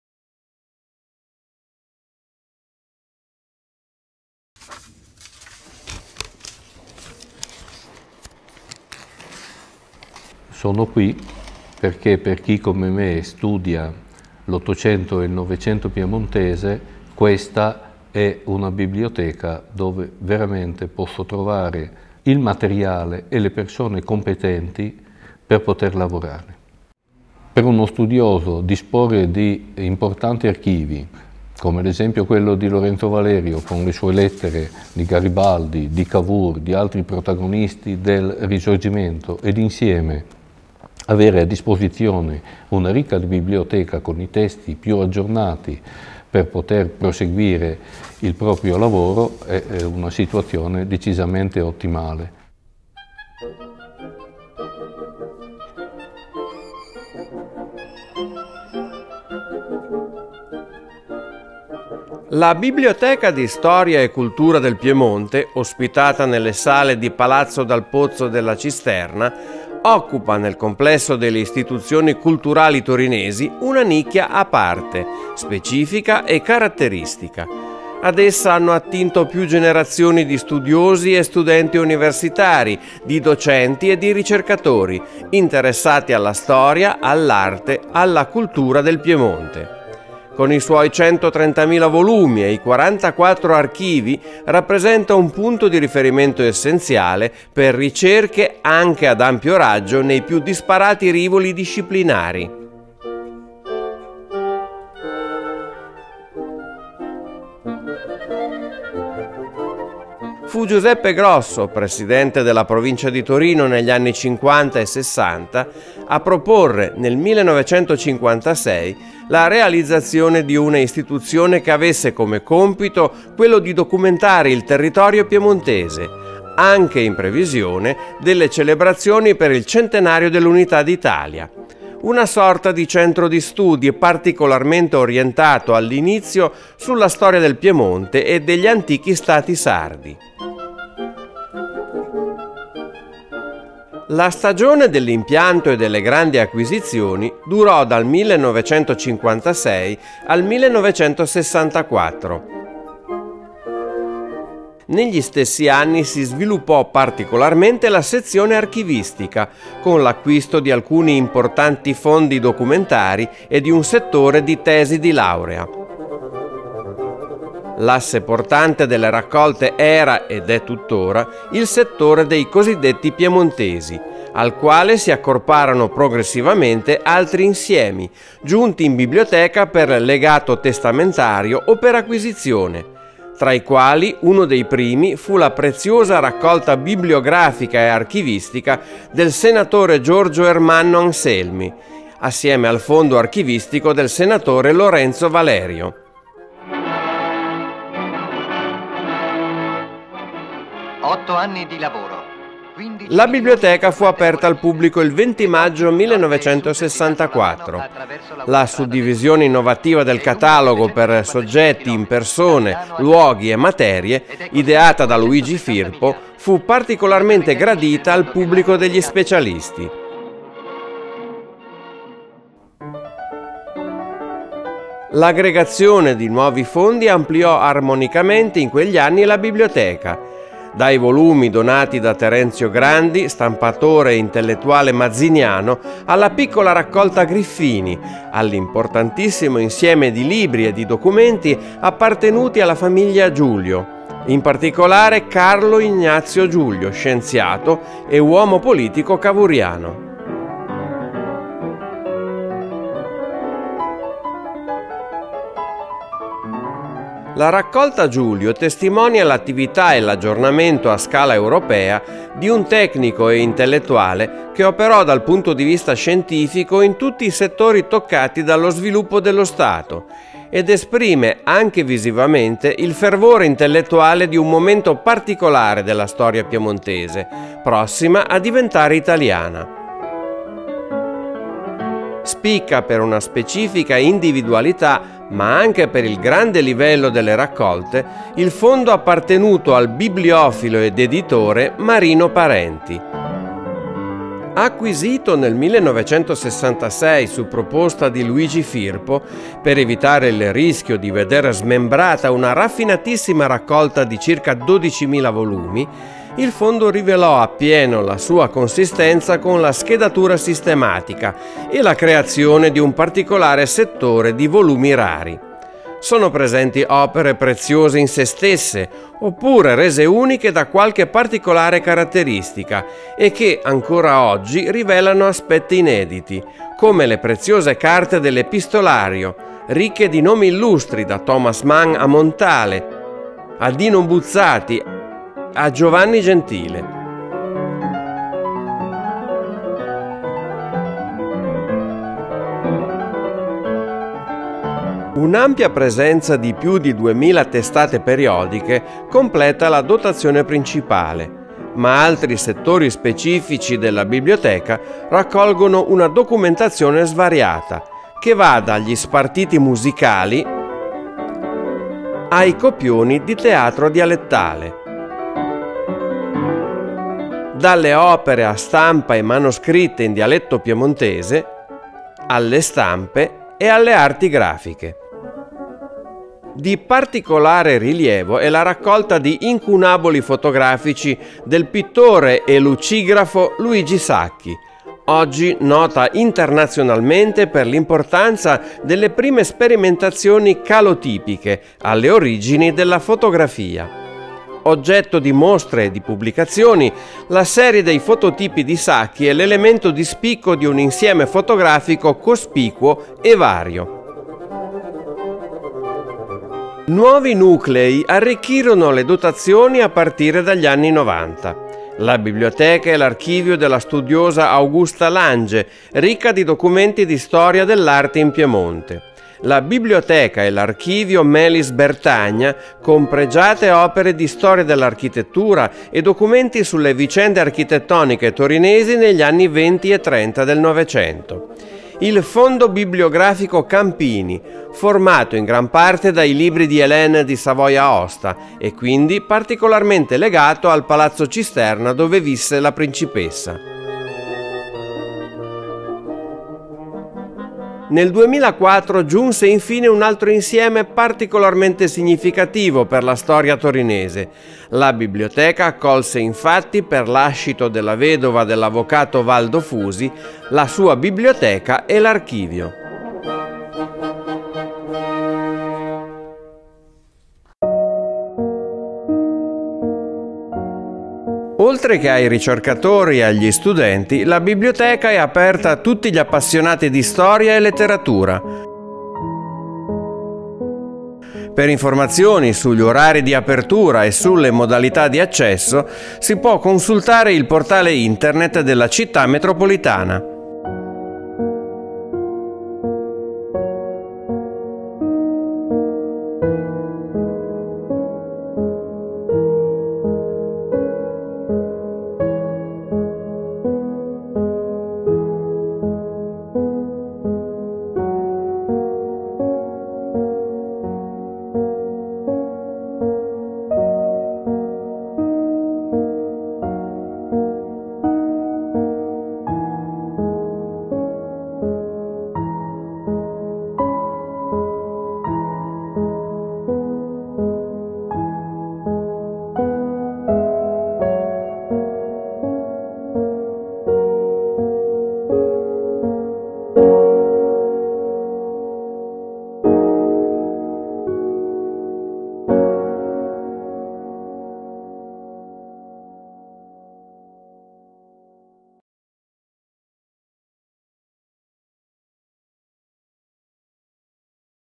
La biblioteca di storia e cultura del Piemonte
La Biblioteca, ospitata nelle sale di palazzo Cisterna, sede della Città metropolitana, con i suoi 130.000 volumi e i 44 archivi rappresenta un punto di riferimento per ricercatori, studenti e appassionati.